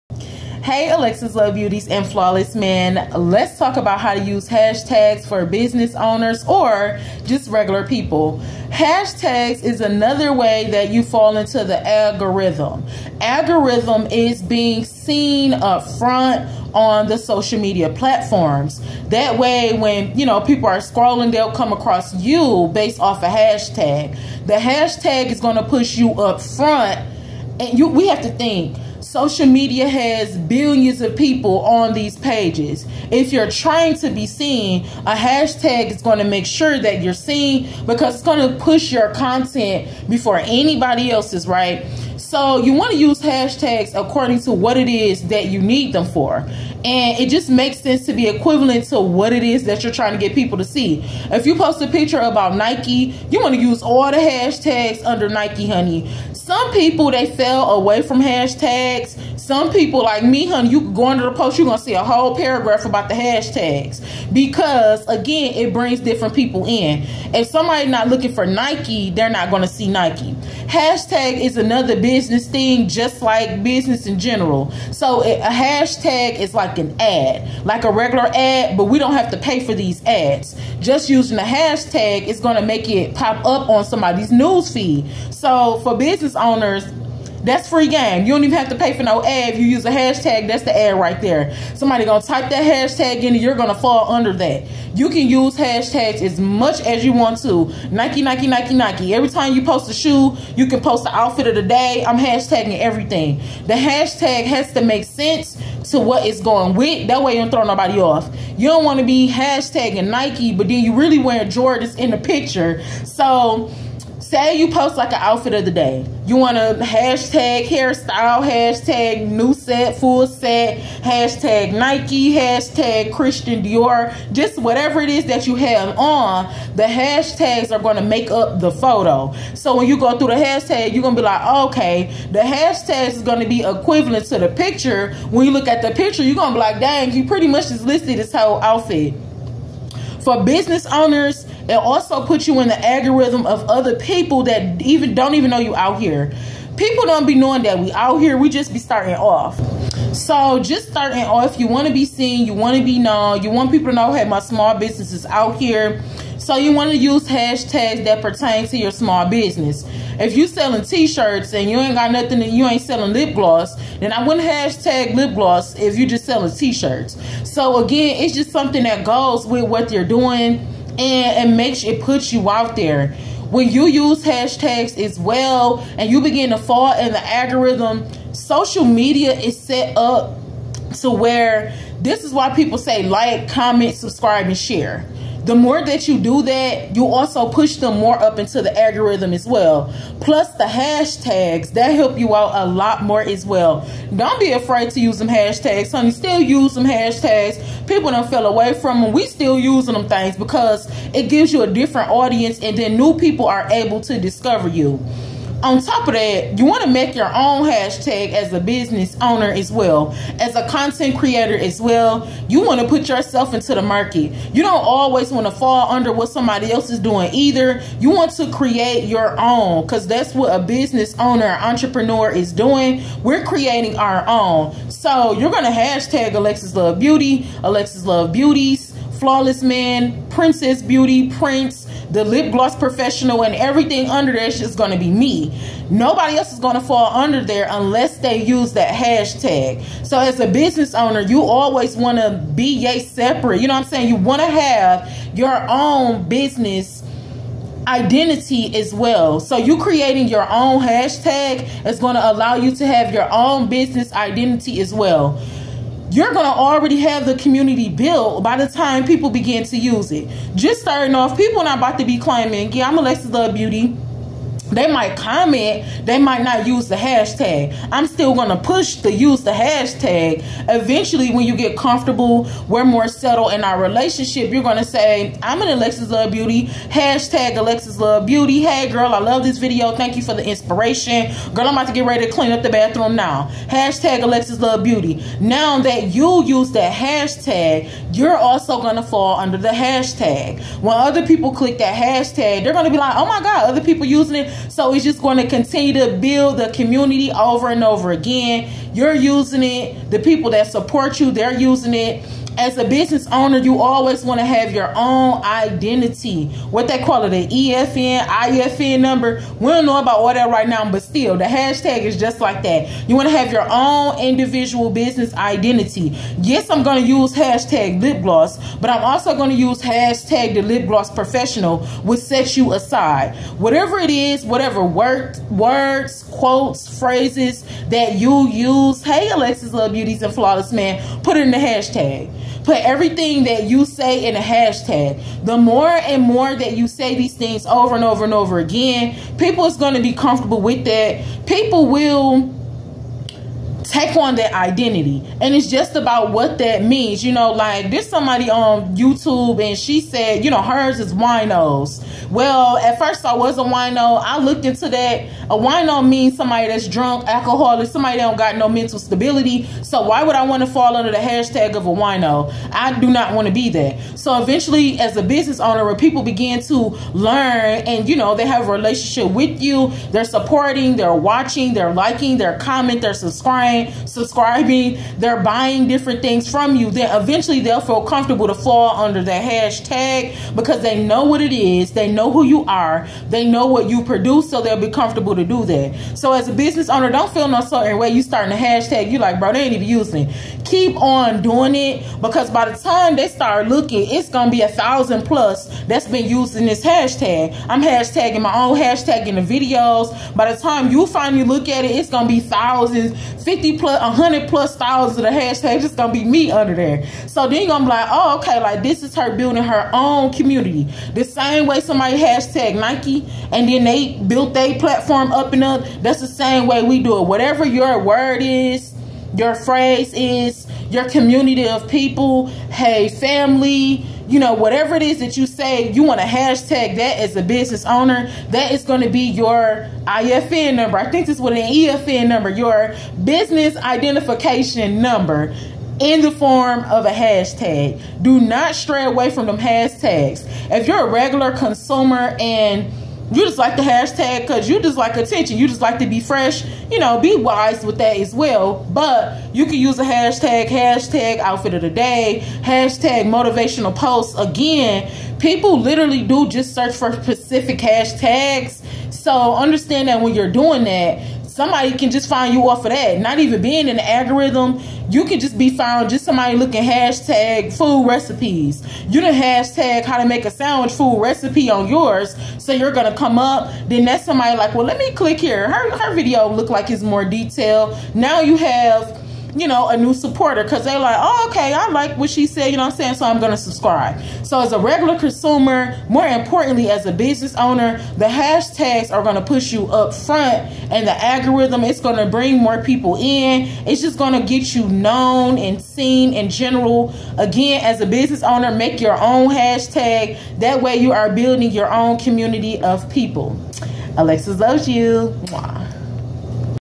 howto Use hashtags on Soical Media 🖥
Hey Alexus Love Beauties and Flawless Men , if you are a Business owner using hashtags are a great way to attract new supporters. Creating your own hashtags like alexuslovesyou alexuslovebeauties thelipglossprofessional flawlessmen allows you to build you own community and it also ties your business directly back you. As a business owner I beleive it is Wise to hashtag whatever popular quote you say that way your Community will have their own identification. A business hashtag is like a Social Media EIN number lol I think that's right. If your a regular consumer and just righthouely want just engage on Social Media use hashtags based on whatever you post so example "The Children and I had an amazing time during our swimming lessons today". swimming swimmingpool learningfun familybonding and so forth. Hashtags are meant to be used for whatever you post and don't be afraid , on some social media platforms you might have up to 5,000 characters to type use all the hashtags you want and No one can stop you 💋